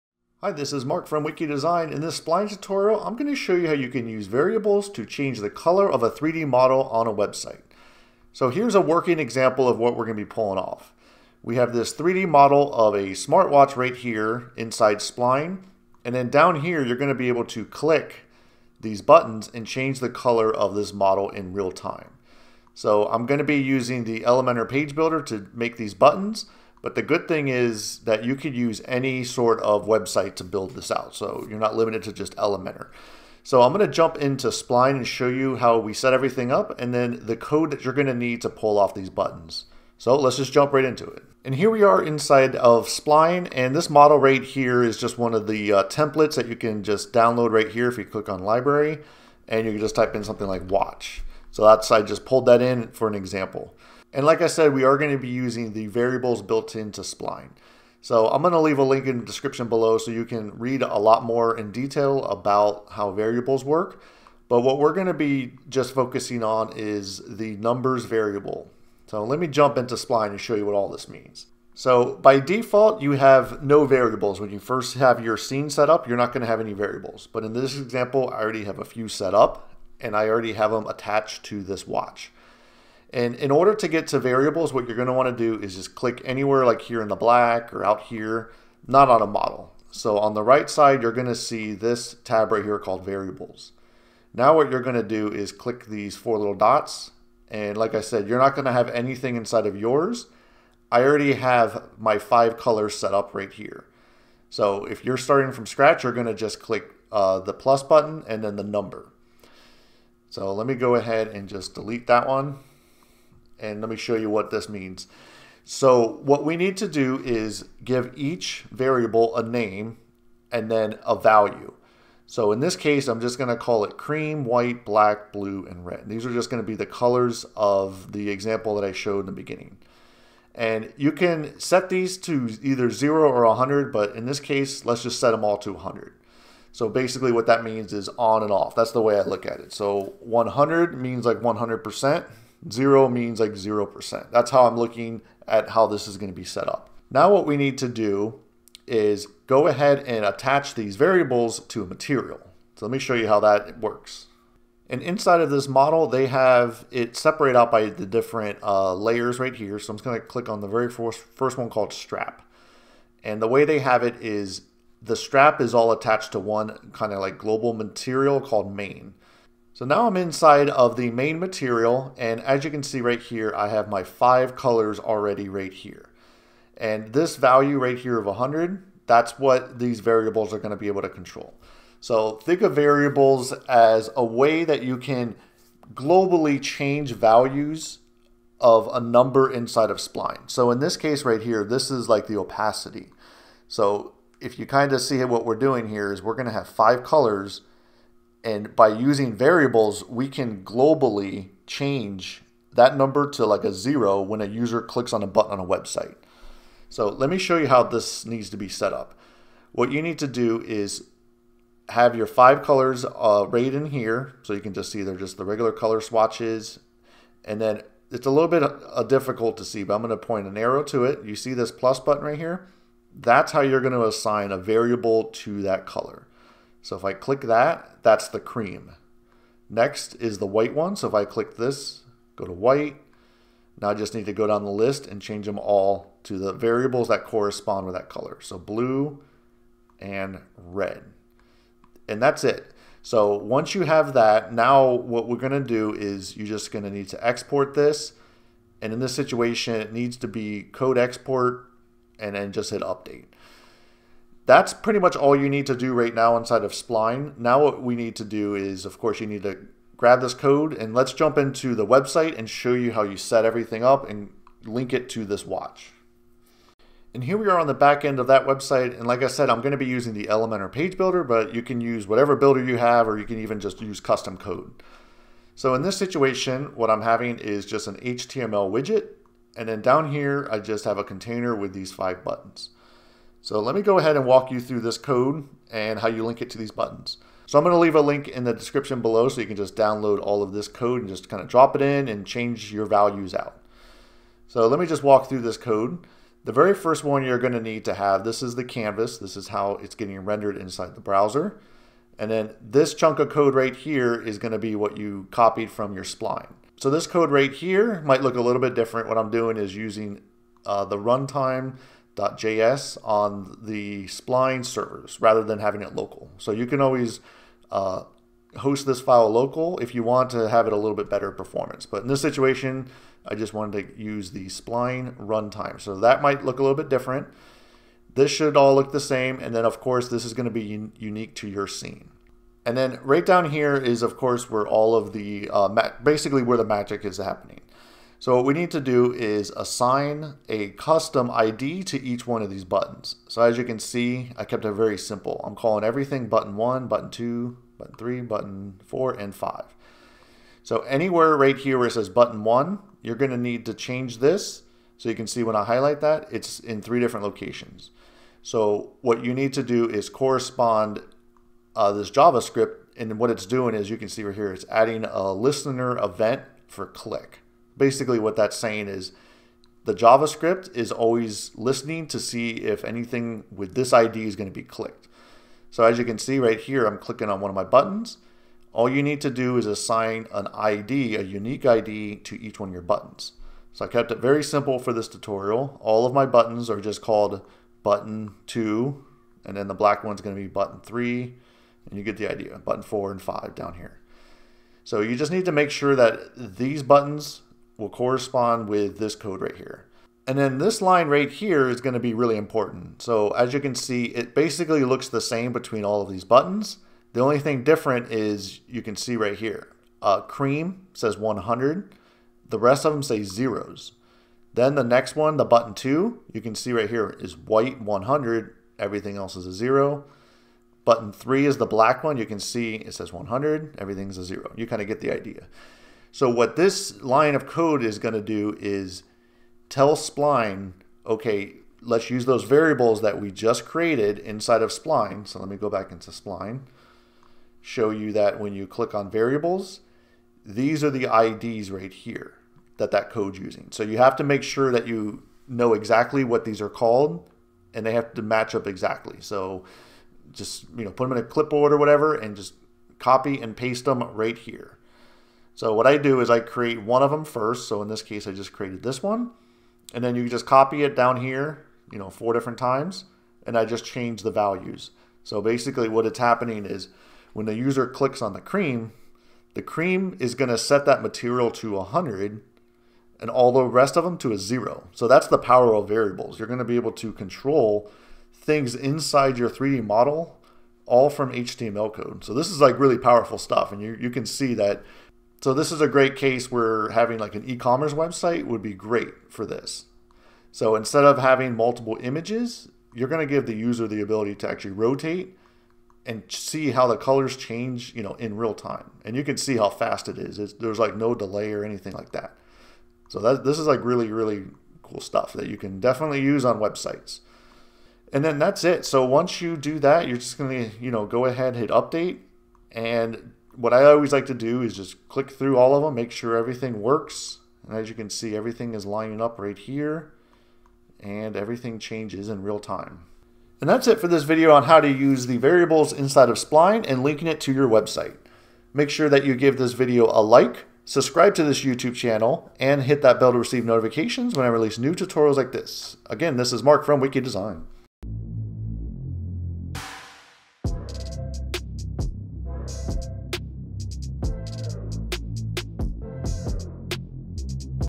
Change 3D Model Textures with HTML (Spline Tutorial)
In this Spline tutorial I will show you how to use variables to change the color of a 3D model from a website.

Timestamps:
0:00 Introduction
0:41 Create Spline Variables
2:34 Assign Variables to Material
4:26 HTML Code Explained